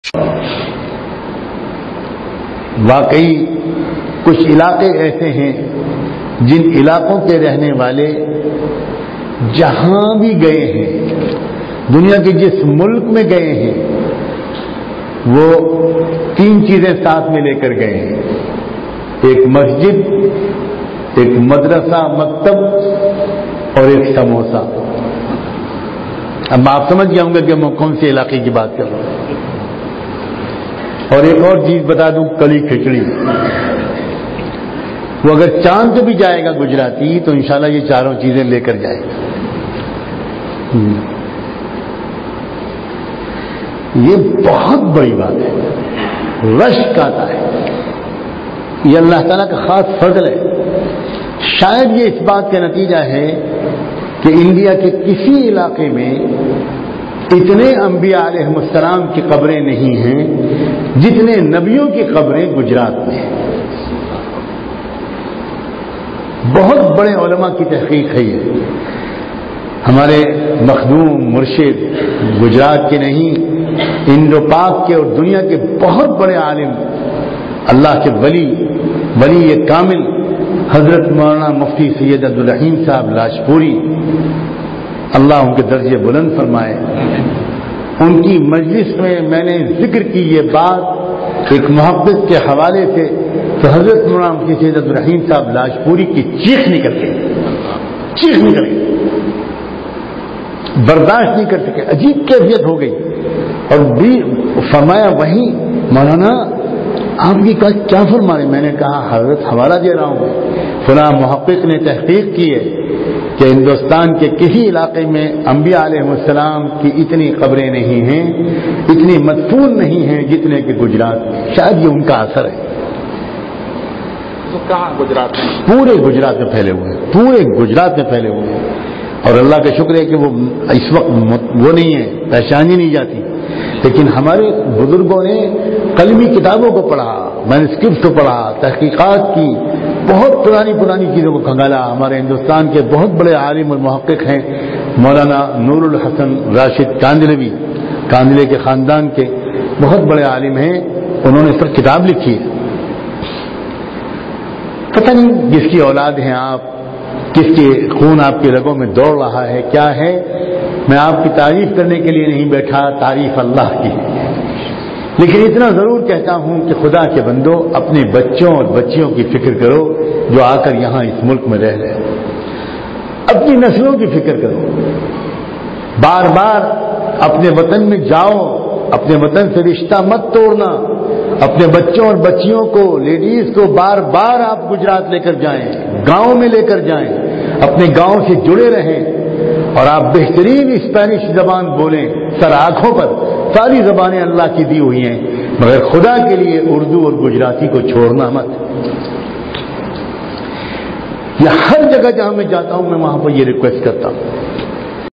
गुजरात के मुस्लमानो केबारे मे बहोत बरीया बात कही हे मोलाना सज्जाद नोमानी साहाब ने सुने ओर आगे शेयर
Video from ✍हक्की आवाज